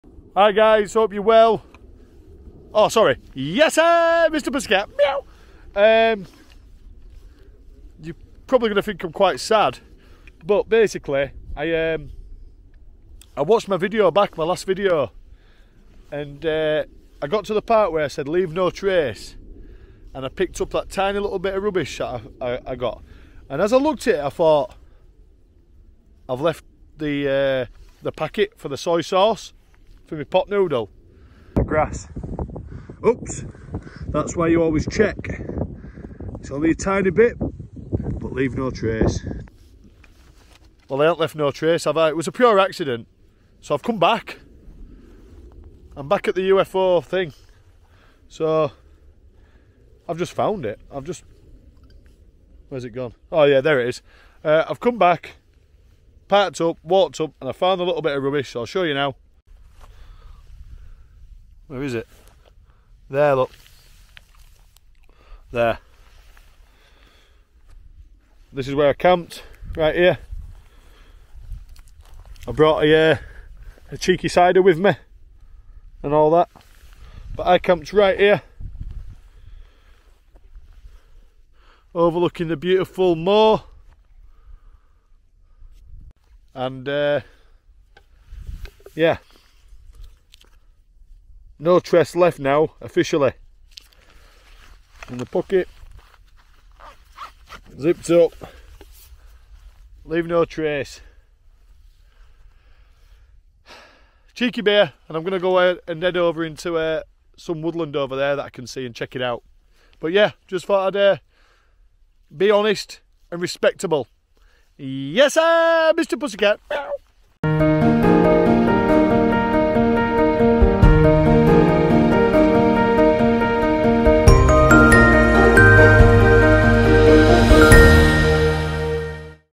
I LEFT TRACE 🙁 - Correcting my MISTAKE!
I noticed on a previous video that I accidentally left a small wrapper, a complete accident!

Thankfully I managed to retrieve it & take the wrapper away with me!